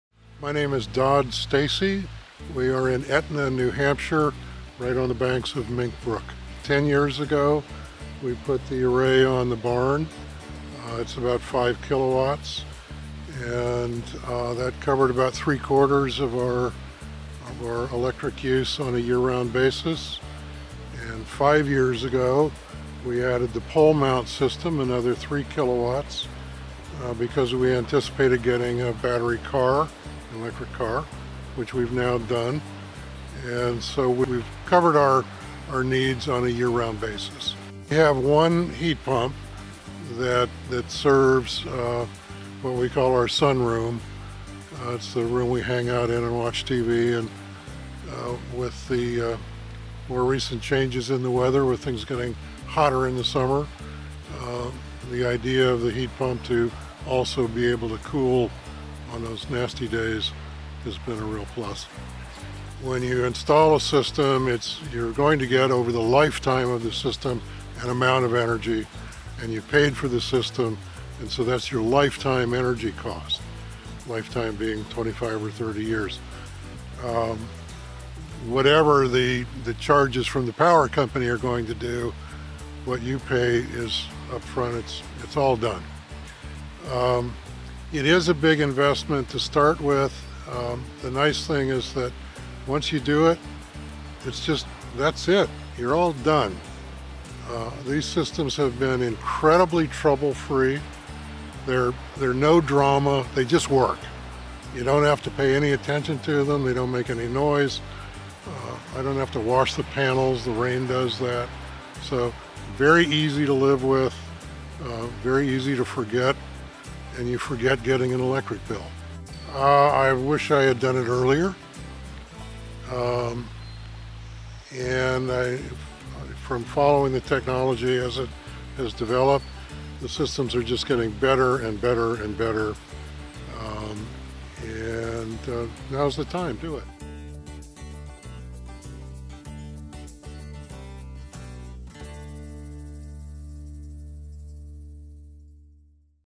Solar, Heat Pump, Electric Vehicle! Dodd Stacy, Etna, NH
Dodd Stacy talks about the powerful combination of clean energy forces - going solar, switching from oil to heat pumps, and gasoline to an electric vehicle... all powered by the sun.